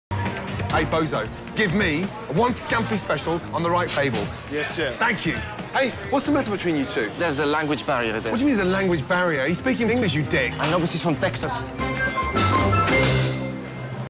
I Know But Hes From Texas :') - Funny Videos Youtube 2022
scene from one of the gordon ramsey shows